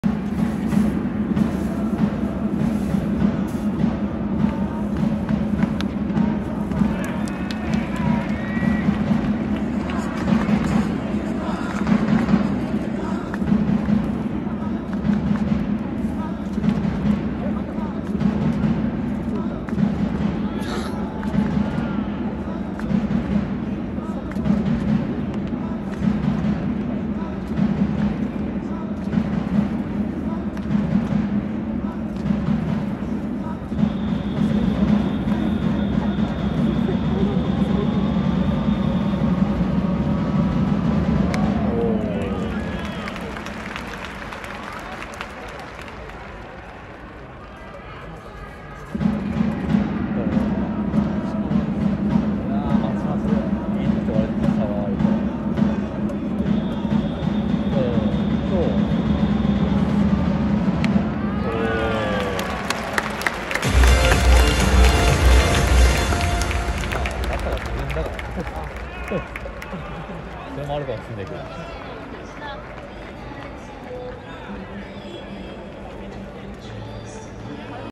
エスコンフィールド北海道埼玉西武蛭間拓哉選手
2023年3月15日エスコンフィールド北海道にて行われた北海道日本ハムファイターズ対埼玉西武ライオンズ戦
蛭間拓哉選手の打席